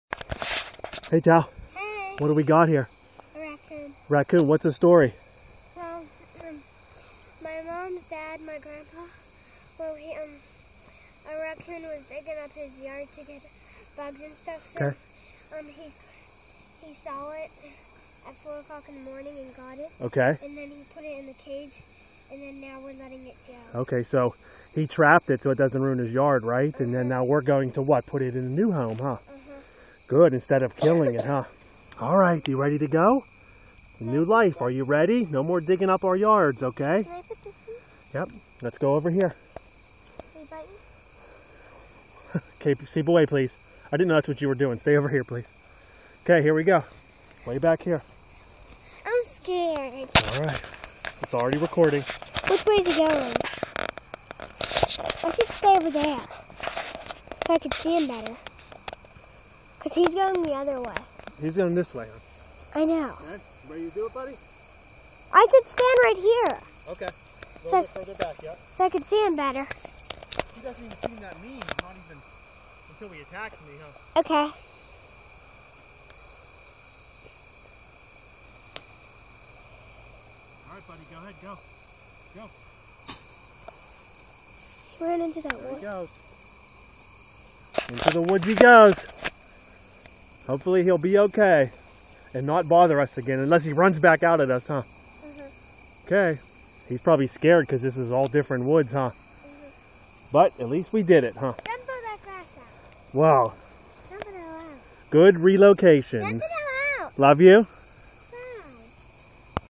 Raccoon box trap story
Catch and release and relocation of Raccoon . These are the things I love to teach my children. I dont care if it's just a varmint.